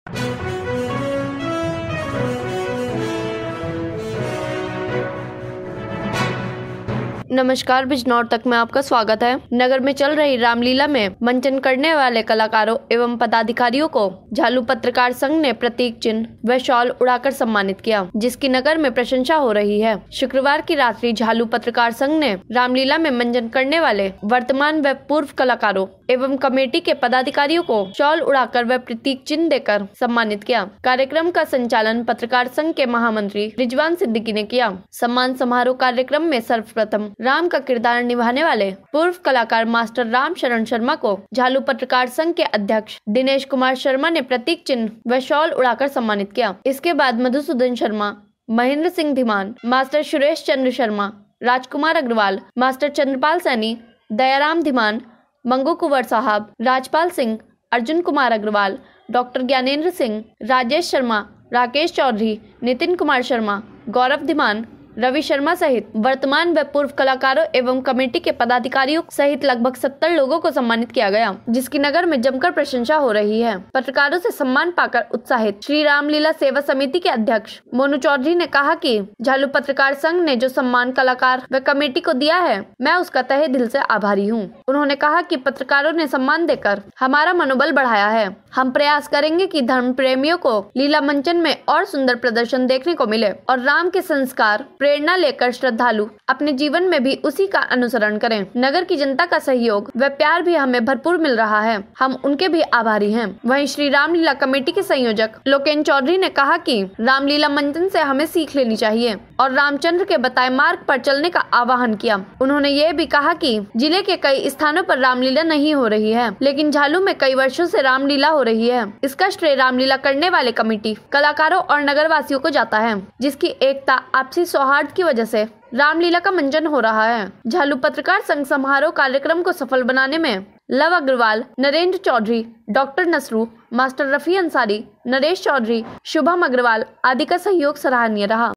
BIJNOR TAK || झालू-रामलीला में मंचन कलाकारों का पत्रकार संघ ने शॉल उड़ाकर सम्मानित किया
झालू। नगर में चल रही  रामलीला में मंचन करने वाले कलाकारों एवं पदाधिकारियों को झालू पत्रकार संघ ने प्रतीक चिन्ह व शाल  उड़ाकर  सम्मानित किया ।  जिसकी नगर में प्रशंसा हो रही है। शुक्रवार की रात्रि झालू पत्रकार संघ ने रामलीला में मंचन करने वाले वर्तमान  व पूर्व कलाकारों  एवं  कमेटी के पदाधिकारियों  को शॉल उड़ाकर व प्रतीक चिन्ह देकर सम्मानित किया। कार्यक्रम का संचालन पत्रकार संघ के  महामंत्री रिजवान सिद्दीकी ने किया। सम्मान समारोह कार्यक्रम में  सर्वप्रथम राम का किरदार निभाने वाले पूर्व कलाकार मास्टर राम शरण शर्मा को  झालू पत्रकार संघ के अध्यक्ष दिनेश कुमार शर्मा ने  प्रतीक चिन्ह व शाल उड़ाकर सम्मानित किया इसके बाद मधुसूदन शर्मा, महेंद्र सिंह धीमान,  मास्टर सुरेश चंद्र शर्मा, राजकुमार अग्रवाल, मास्टर चंद्रपाल सैनी, दयाराम धीमान , मंगू कुंवर साहब ,राजपाल सिंह, अर्जुन कुमार अग्रवाल, डॉ ज्ञानेंद्र सिंह, राजेश शर्मा ,राकेश चैधरी, नितिन कुमार शर्मा, गौरव धीमान ,रवि शर्मा सहित वर्तमान व पूर्व कलाकारों एवं  कमेटी के  पदाधिकारियों सहित लगभग 70 लोगों को  सम्मानित किया गया । जिसकी नगर में  जमकर प्रशंसा हो रही है । पत्रकारों से सम्मान पाकर  उत्साहित श्री रामलीला सेवा समिति के अध्यक्ष मोनू चैधरी ने कहा कि झालू पत्रकार संघ ने जो सम्मान कलाकारों व कमेटी को दिया है, मैं उसका तहे दिल से आभारी हूं। उन्होंने कहा कि  पत्रकारों ने  सम्मान देकर हमारा मनोबल बढ़ाया है, हम प्रयास करेंगे की धर्म प्रेमियों को लीला मंचन में और सुंदर प्रदर्शन देखने को मिले और राम के संस्कार प्रेरणा लेकर श्रद्धालु अपने जीवन में भी उसी का अनुसरण करें, नगर  की जनता का सहयोग  वह प्यार भी हमें भरपूर मिल रहा है । हम उनके भी आभारी हैं । वही श्री रामलीला कमेटी के संयोजक लोकेंद्र चैधरी ने कहा कि रामलीला मंचन से हमें सीख लेनी चाहिए और राम चंद्र के बताए मार्ग पर चलने का आह्वान किया। उन्होंने ये भी कहा कि जिले के कई स्थानो पर रामलीला नहीं हो रही है, लेकिन झालू में कई वर्षों से रामलीला हो रही है। इसका श्रेय रामलीला करने वाले कमेटी, कलाकारों और नगरवासियों को जाता है। जिसकी एकता आपसी सौहार्द की वजह  से  ही रामलीला का मंचन  हो रहा है। झालू पत्रकार संघ सम्मान समारोह कार्यक्रम को सफल बनाने में लव अग्रवाल ,नरेंद्र चैधरी, डॉक्टर  नसरू, मास्टर रफी अंसारी ,नरेश चैधरी ,शुभम अग्रवाल आदि का सहयोग सराहनीय रहा।